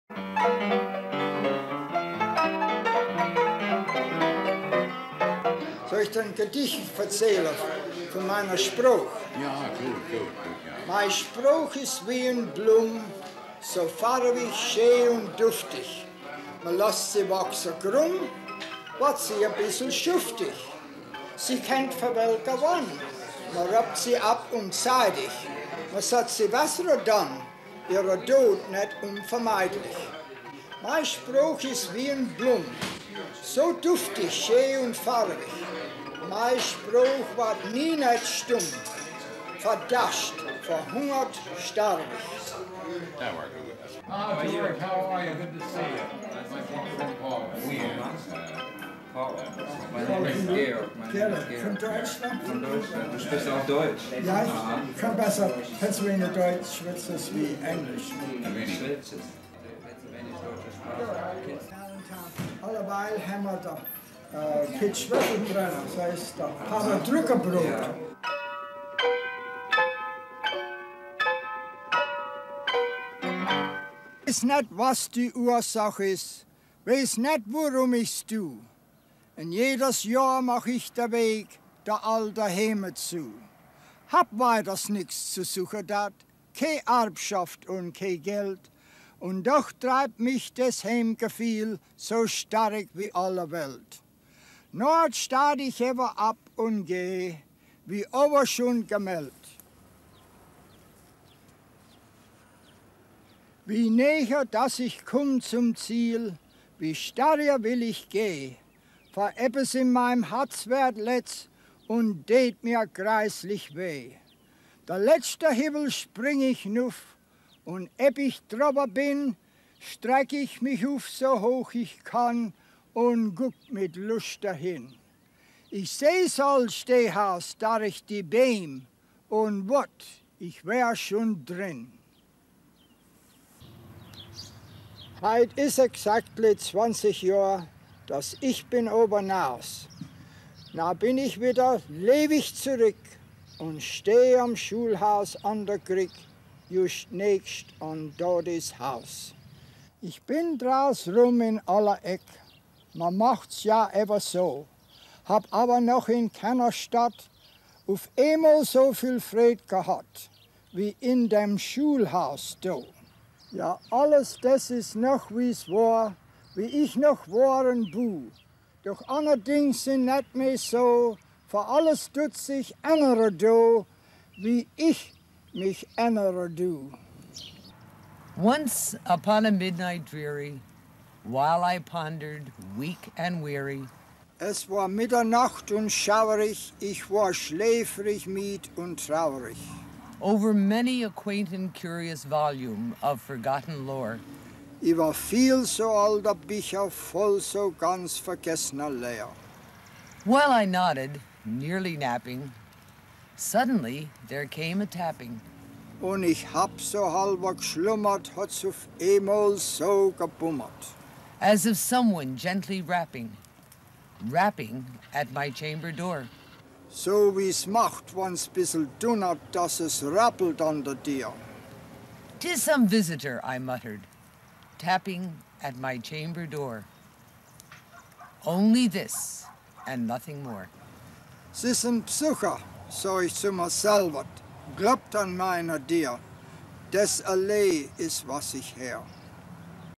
Pennsylvania Dutch poetry | Pennsylvania German documentary
Paul R. Wieand from Allentown shares poems in the beautiful Pennsylvania Dutch language.